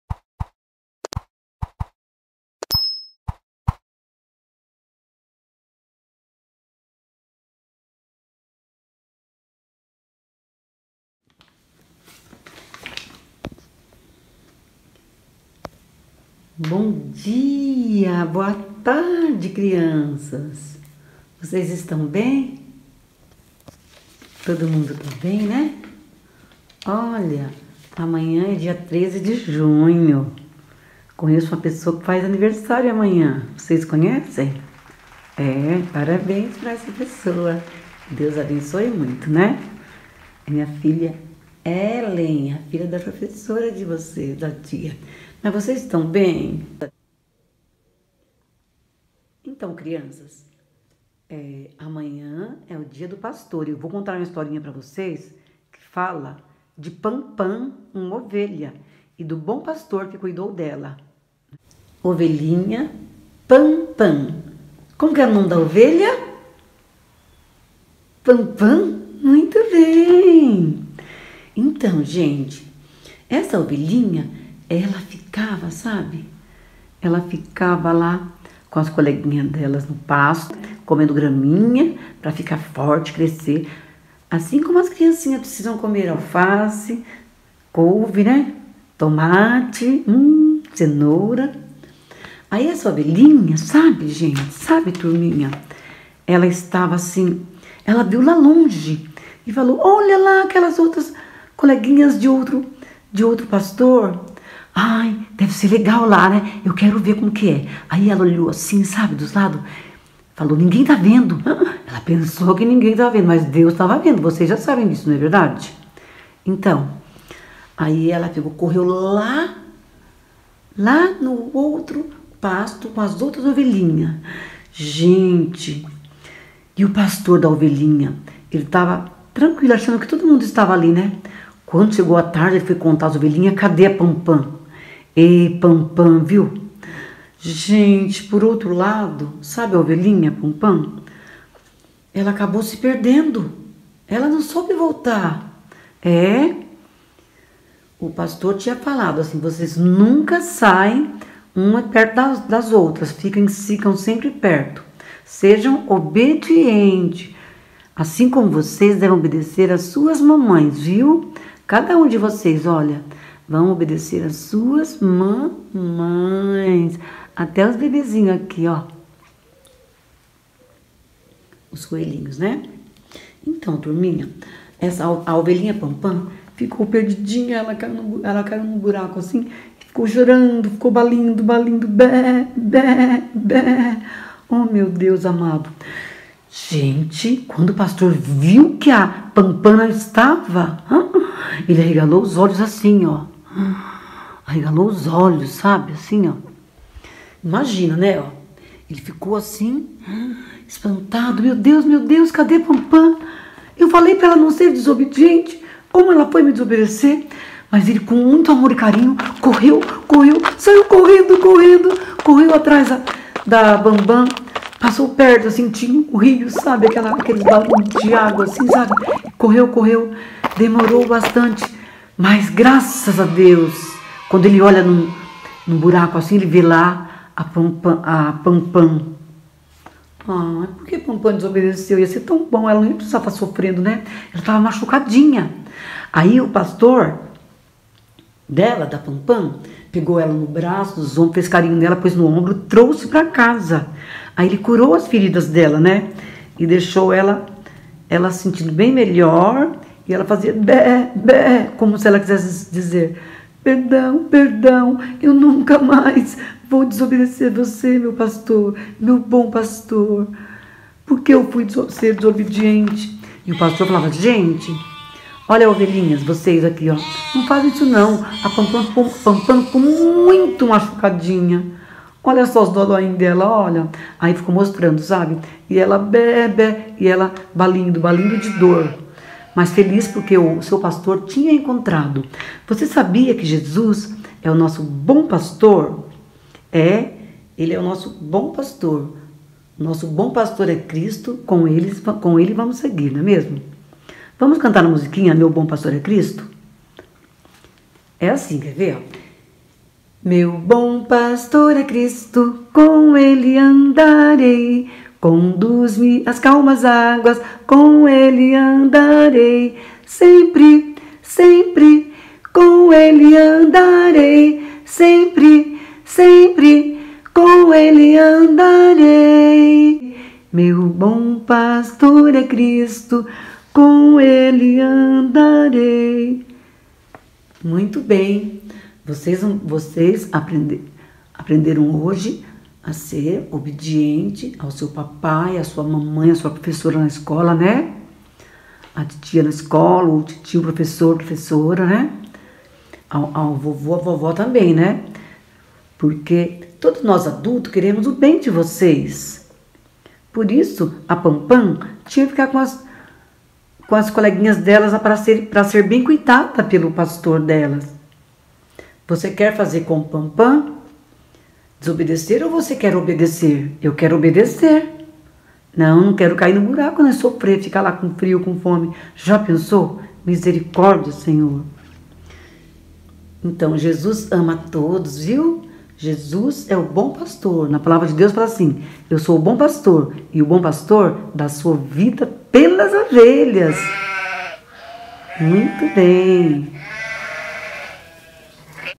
PAMPAM A OVELHINHA. Para você mamãe contar essa história para as crianças.
PAMPAM A🐑

Vós, filhos, sede obedientes a vossos pais no Senhor, porque isto é justo. 2 Honra a teu pai e a tua mãe, que é o primeiro mandamento com promessa, 3 para que te vá bem, e vivas muito tempo sobre a terra.

4 E vós, pais, não provoqueis a ira a vossos filhos, mas criai-os na doutrina e admoestação do Senhor.

5 Vós, servos, obedecei a vosso senhor segundo a carne, com temor e tremor, na sinceridade de vosso coração, como a Cristo, 6 não servindo à vista, como para agradar aos homens, mas como servos de Cristo, fazendo de coração a vontade de Deus; 7 servindo de boa vontade como ao Senhor e não como aos homens, 8 sabendo que cada um receberá do Senhor todo o bem que fizer, seja servo, seja livre. 9 E vós, senhores, fazei o mesmo para com eles, deixando as ameaças, sabendo também que o Senhor deles e vosso está no céu e que para com ele não há acepção de pessoas.